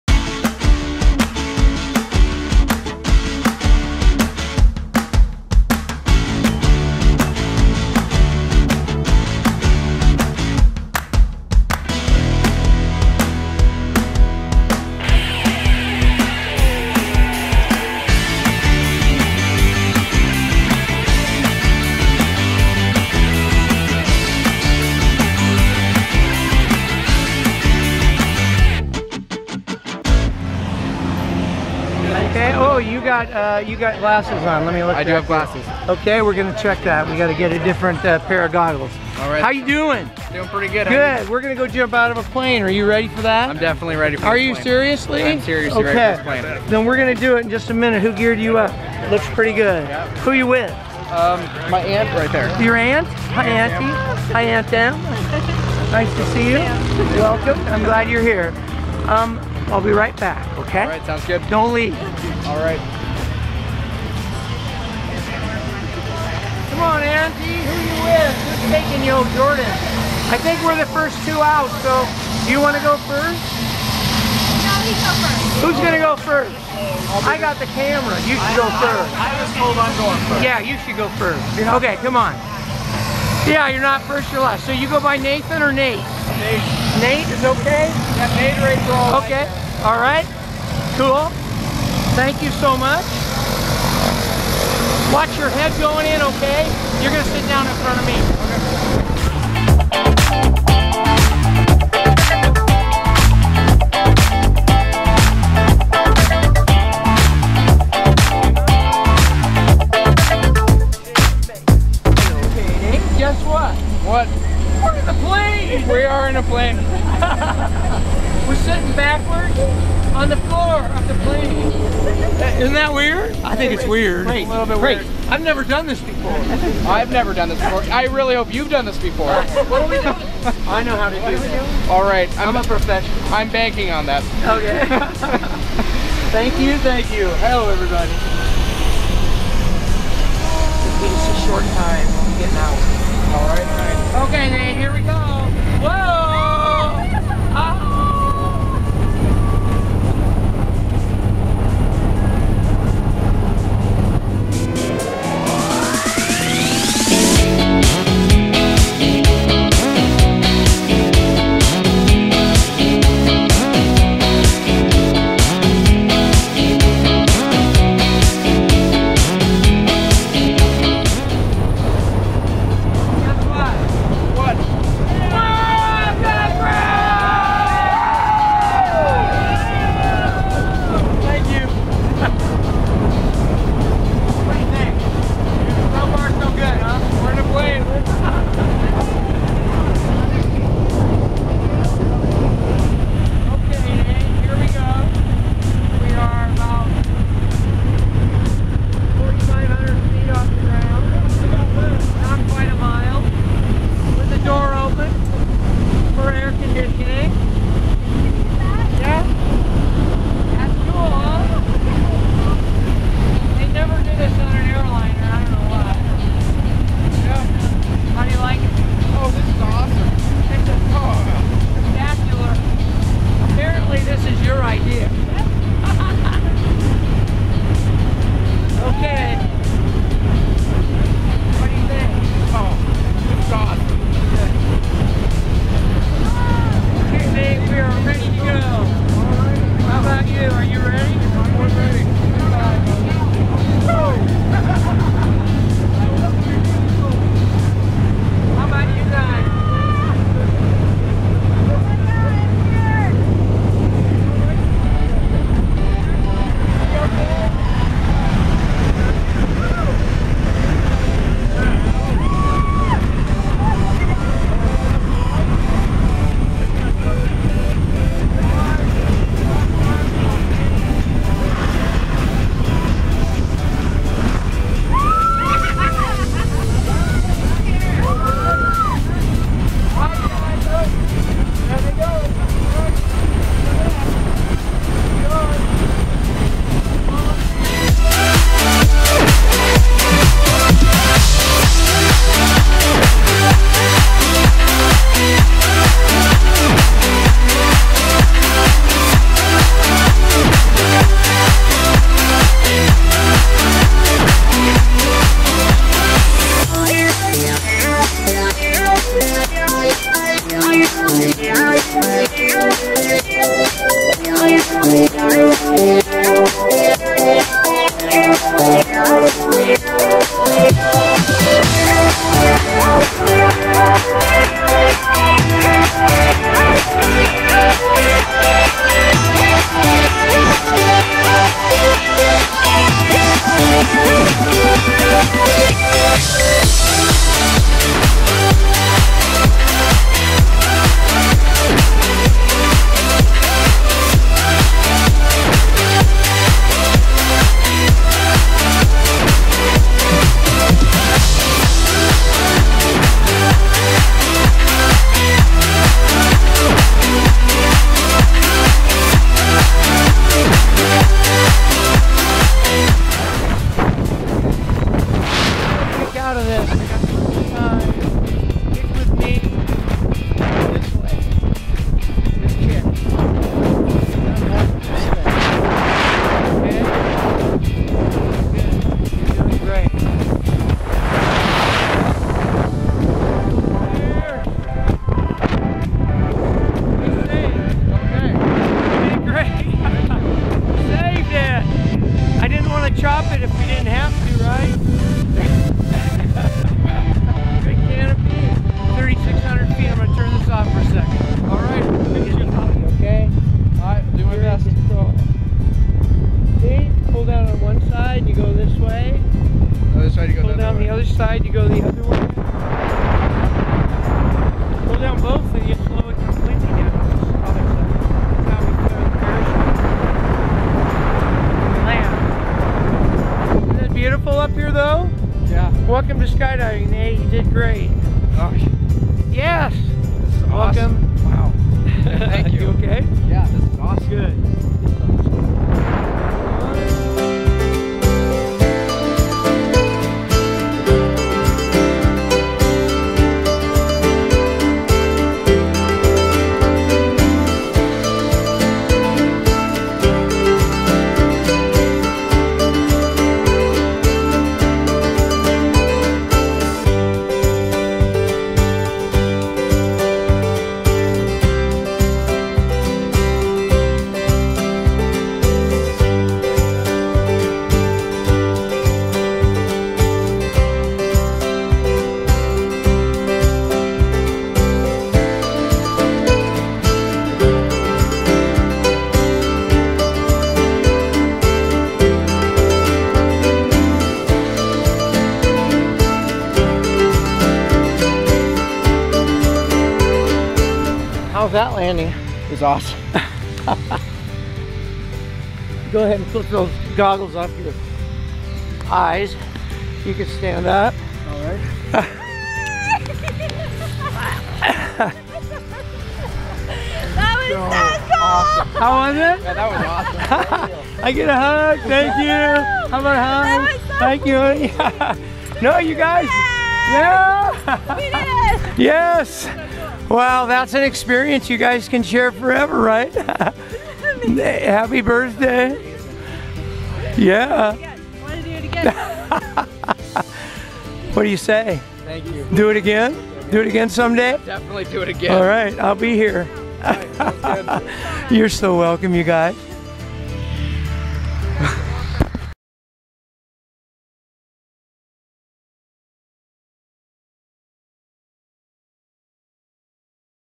Nathan O'Hanlon's Tandem skydive!
Nathan O'Hanlon's tandem skydive at Skydive Snohomish!